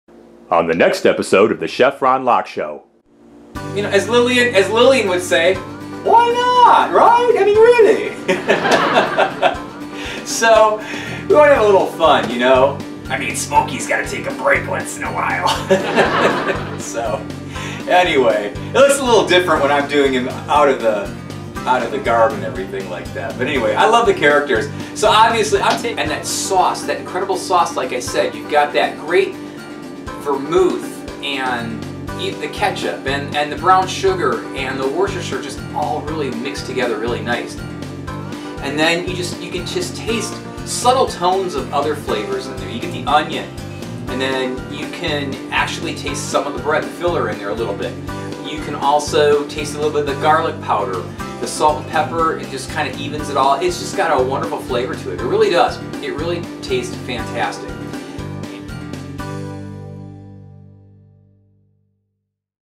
Coming Up This Thursday 7/24 on "The Chef Ron Lock Show" S1/E16
"The Chef Ron Lock Show" is a cross between Saturday Night Live and the Food Network. This show airs Thursdays at 8 PM EDT.

This is the 16th and last episode of Season One of my cooking show.

I decided to complete the first season without any of the characters so you get Chef Ron Lock for the entire episode. This episode deals with a very special recipe attached to some very special personal memories for me. I can't say anymore so you will have to turn into our last episode and see for yourself.

I am presenting a classic main dish that many of you will remember from your childhood years and most likely are still preparing a version of it today!

Come join in on the fun and food only on "The Chef Ron Lock Show"